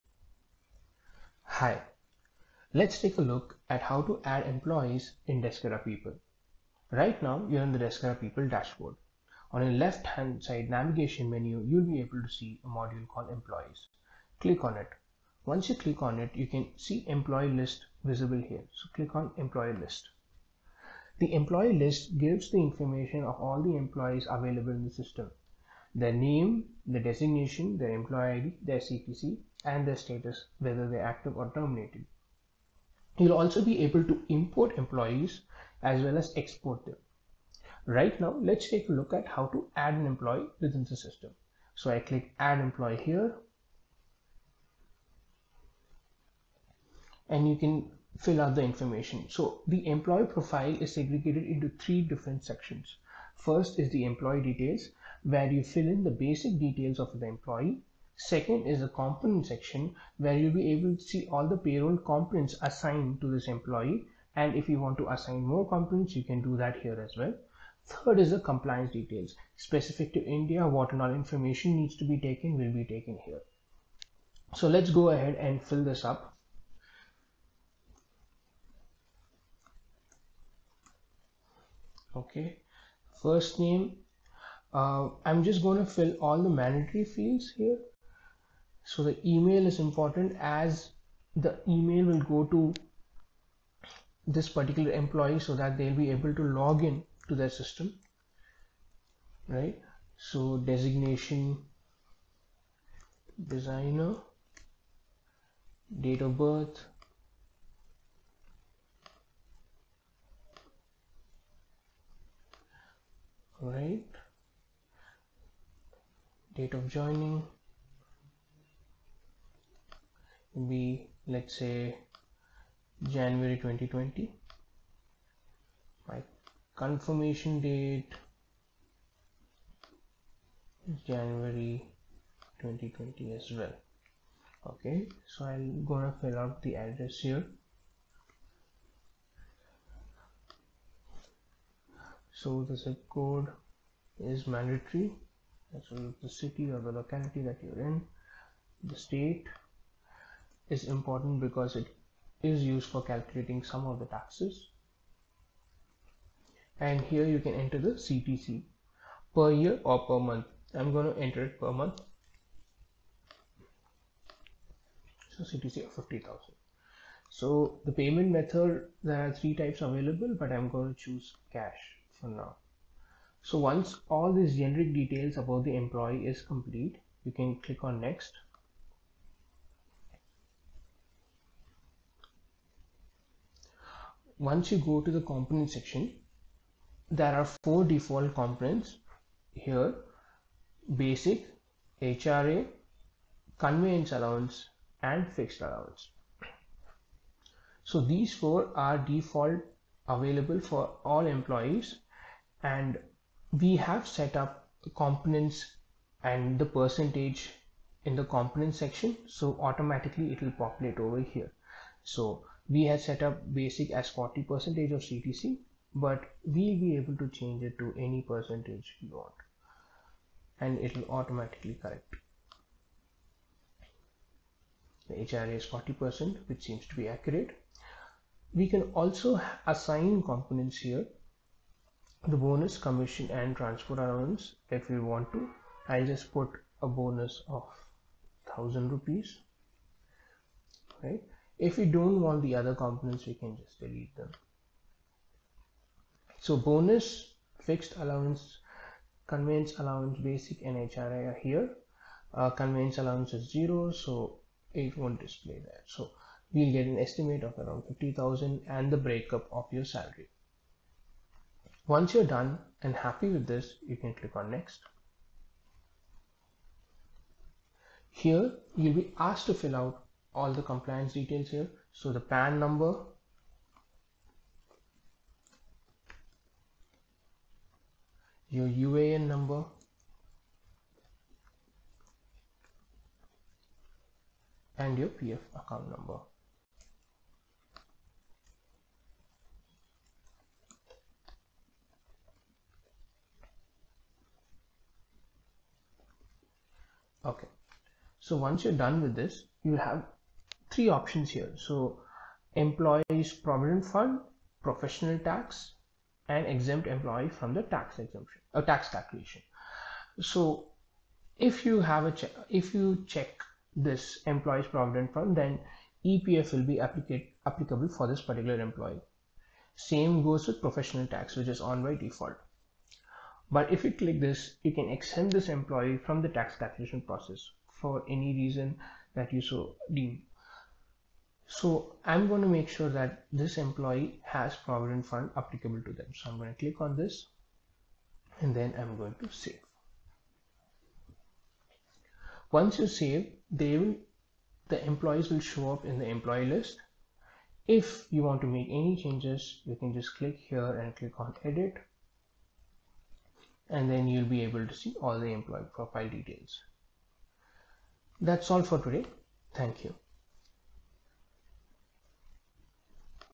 Adding Employees in Deskera People
In this tutorial, we will understand how to add employees and completing their profiles in Deskera People.